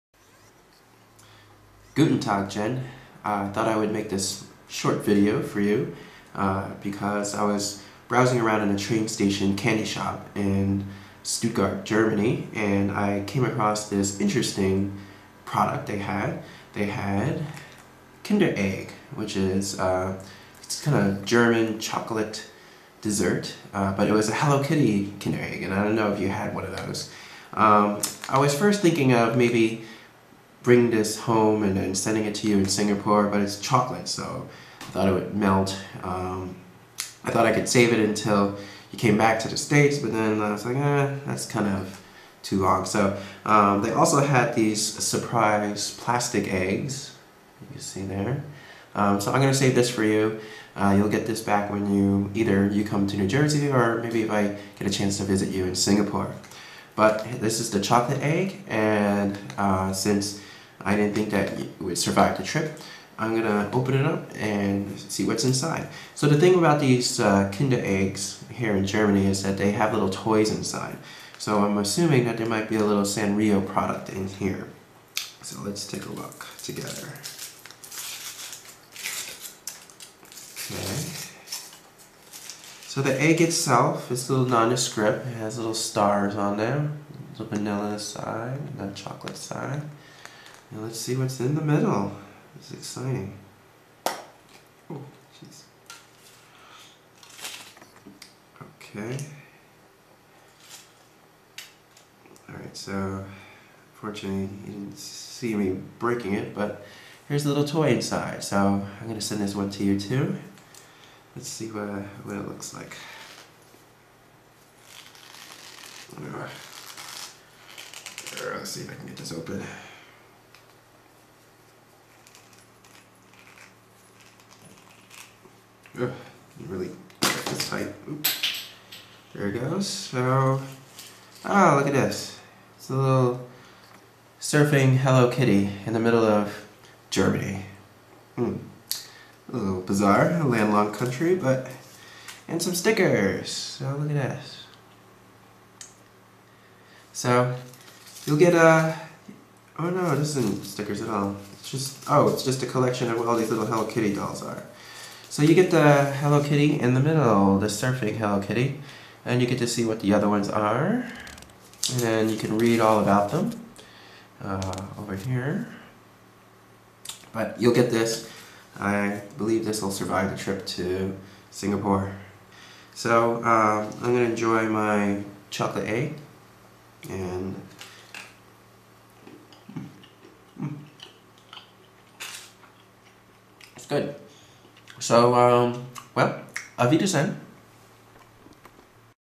Hello Kitty Egg
Warning, this will probably bore anyone who is not my friend Jennifer (and probably her too).

I have no idea why I said I might see her in NJ (she used to live in California) or why I kept on pronouncing Kinder Egg like "Kinda" Egg.

As a bonus, you all now know how powerful and strong I really am!